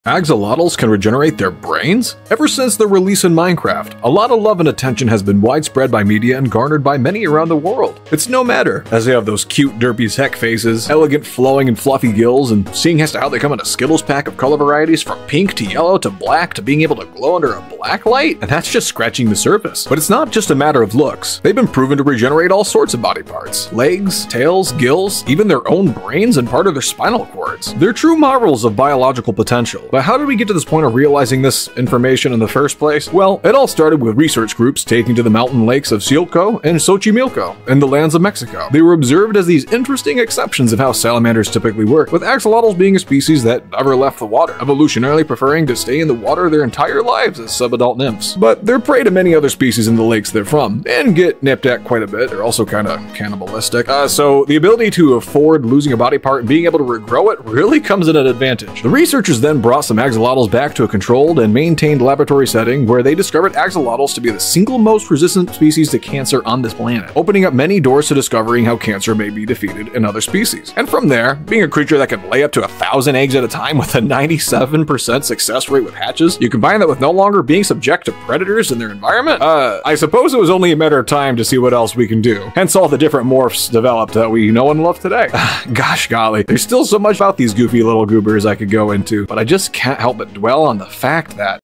They Can Regenerate Their Brains!?
As a big fan, and considering how many already know and love axolotls, it was only a matter of time before I go into these marvelous little goofy goobers! Axolotls are fascinating in their biology, not to mention cute as heck! (I know, I know, it's subjective taste.) Learn more about their ability to regenerate body parts and more!